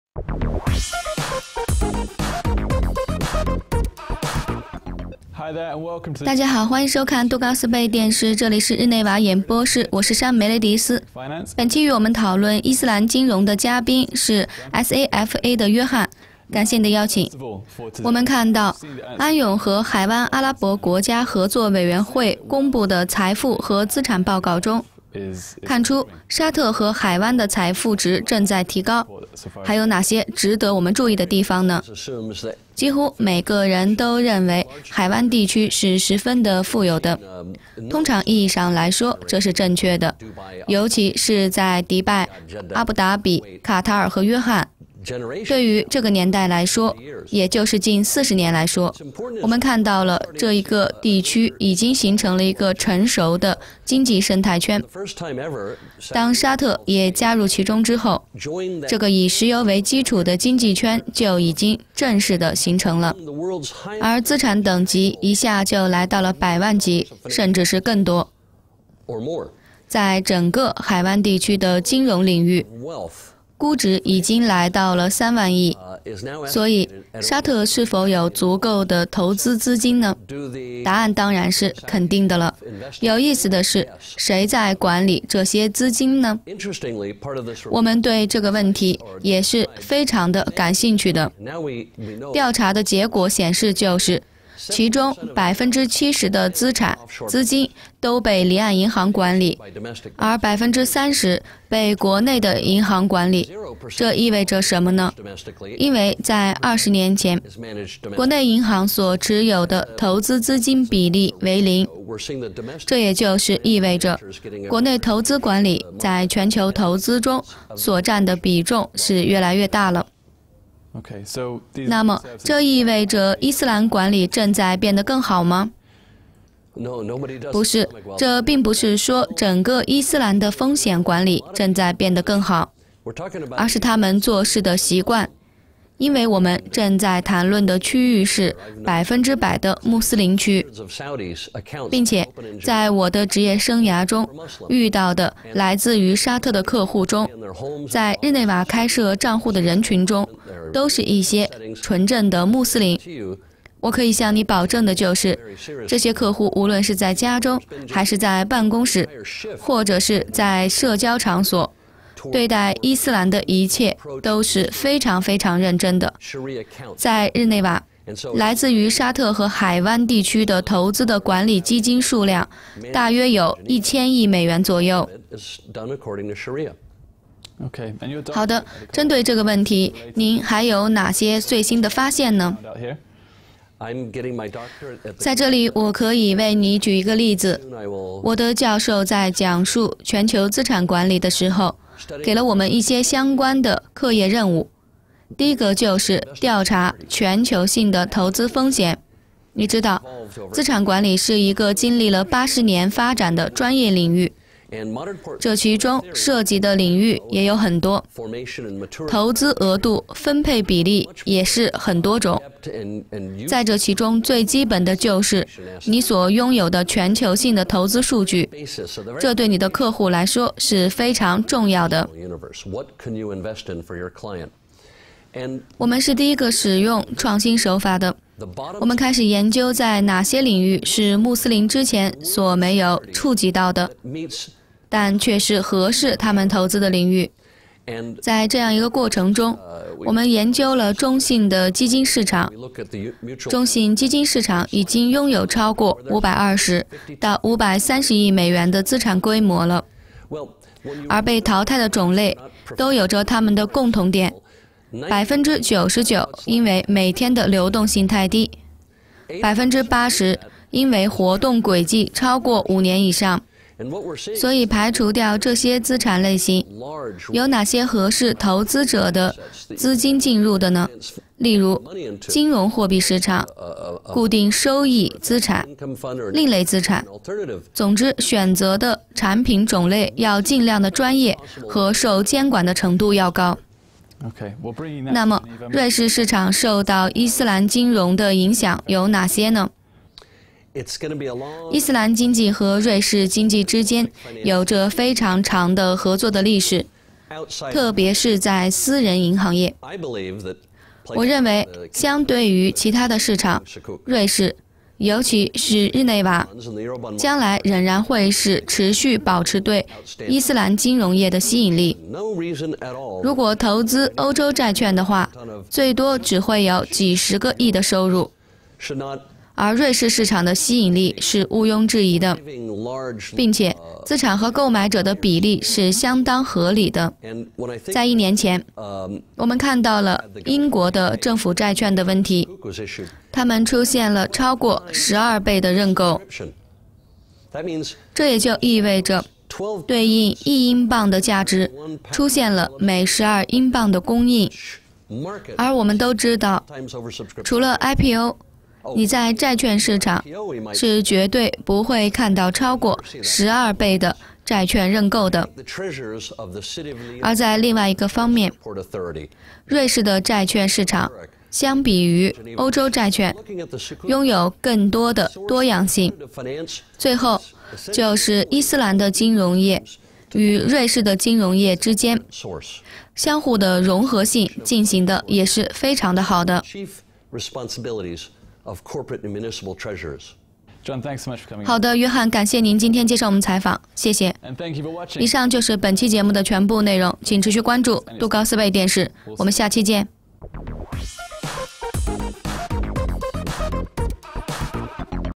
伊斯兰金融
你在债券市场绝不会看到超过12倍的债券认购，约翰·桑威克 ，总经理 ，SAFA Investment services